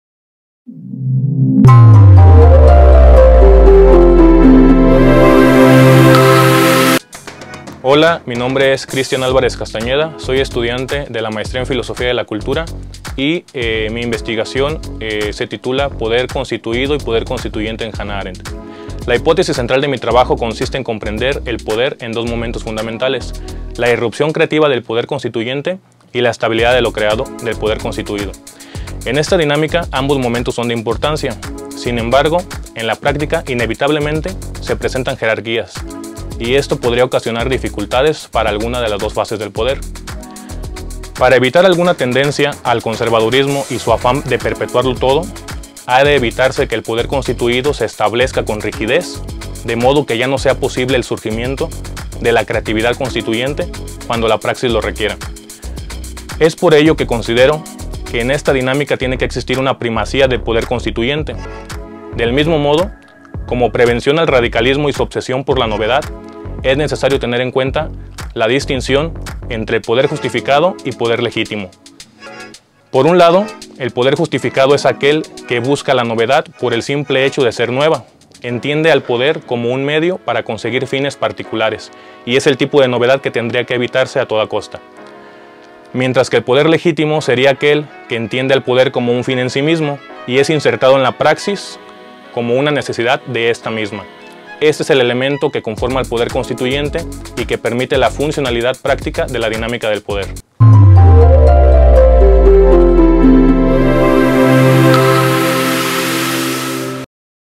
PODER CONSTITUYENTE Y PODER CONSTITUIDO
El poder constituyente es el que dice cuáles son las reglas básicas de la convivencia y cuáles podrán modificarse (y de qué manera) en el futuro y el poder constituido es el que aplica tales reglas, pudiendo innovar dentro del estricto marco establecido por aquel.